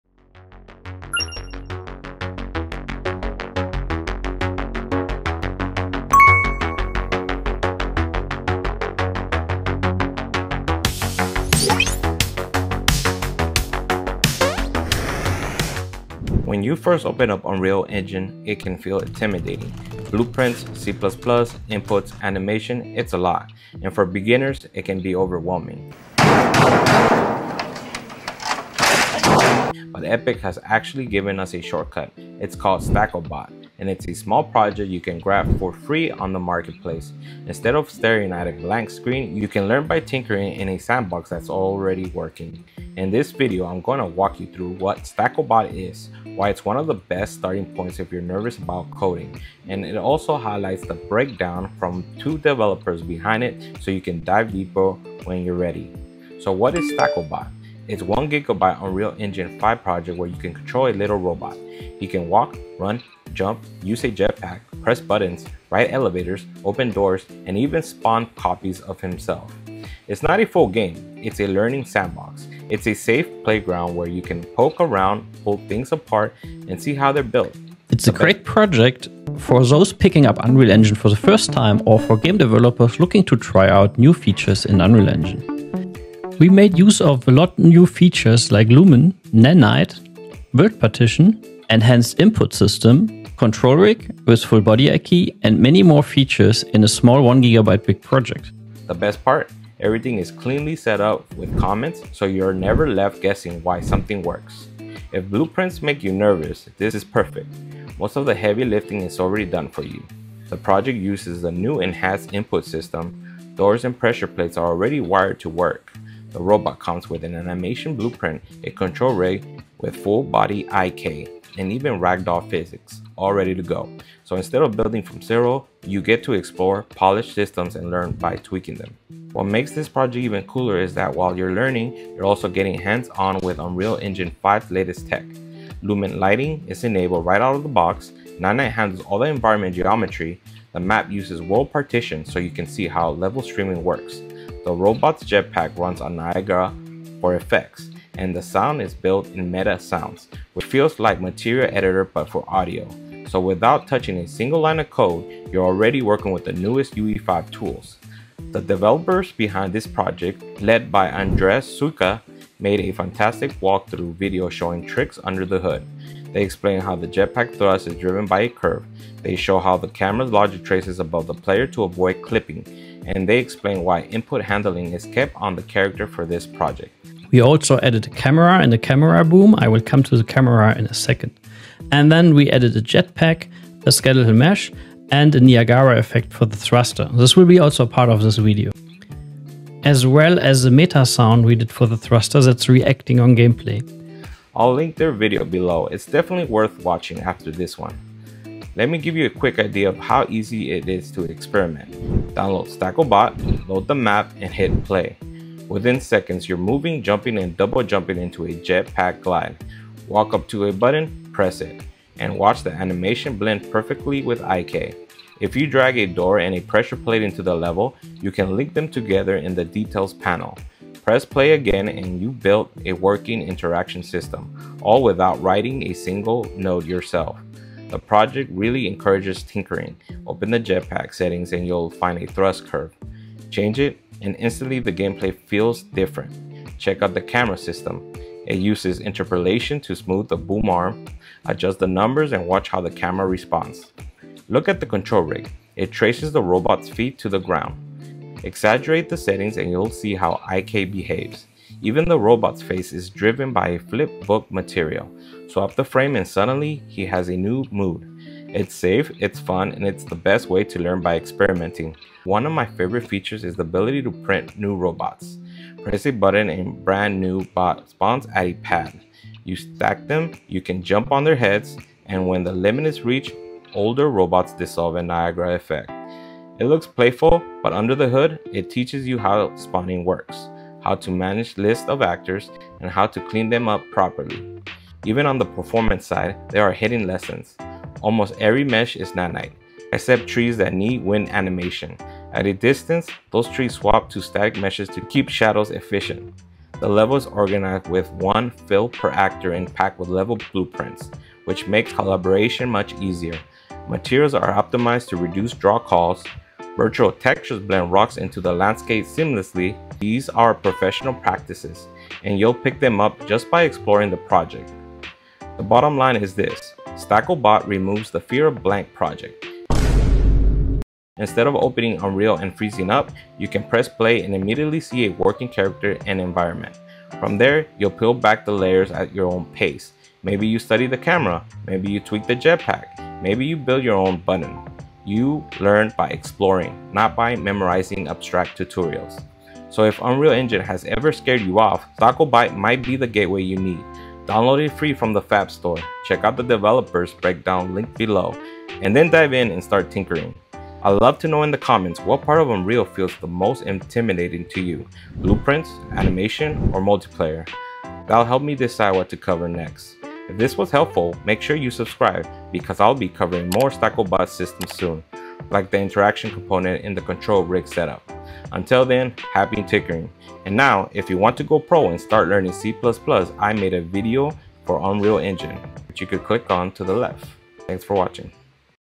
Unreal Engine 5 Doesn't Have to Be Overwhelming
Want to learn Unreal Engine 5 fast but don’t know where to start? In this video, I’ll walk you through Stack-O-Bot, Epic’s free beginner-friendly project that makes learning UE5 simple and fun. Stack-O-Bot is a small sandbox that comes with a playable robot, jetpack movement, interactive doors, buttons, and puzzles — all powered by Unreal Engine’s latest features like Lumen, Nanite, World Partition, Control Rig, Niagara, and MetaSounds.

If Blueprints or coding have ever intimidated you, this project is the perfect way to get hands-on without feeling overwhelmed. You’ll see how to explore systems like animation, input mapping, interactions, and optimization — all set up and ready for you to tinker with.

Whether you’re brand new to Unreal or just want a safe starting point, Stack-O-Bot is the best way to learn UE5 by doing.

⏱️ Timestamps – Learn Unreal with Stack-O-Bot

00:00 – Unreal Engine Can Feel Overwhelming
00:29 – Stack-O-Bot: Your Free Beginner Sandbox
01:00 – What You Can Do (Jetpack, IK, Doors)
01:30 – Built-In UE5 Tech (Lumen, Nanite, Input)
02:15 – Dev Walkthrough: Jetpacks, Cameras, Setup
03:17 – Try It Yourself: Learn by Tweaking
03:52 – Gameplay Adjustments & IK Demos
04:21 – Spawn Bots, Actor Lists & Cleanup
04:44 – Hidden Performance Tips (Nanite, LODs)
05:18 – From Blank Project to Real Progress
05:41 – What’s Hardest for You in UE?
06:02 – More Stack-O-Bot & C++ Coming Soon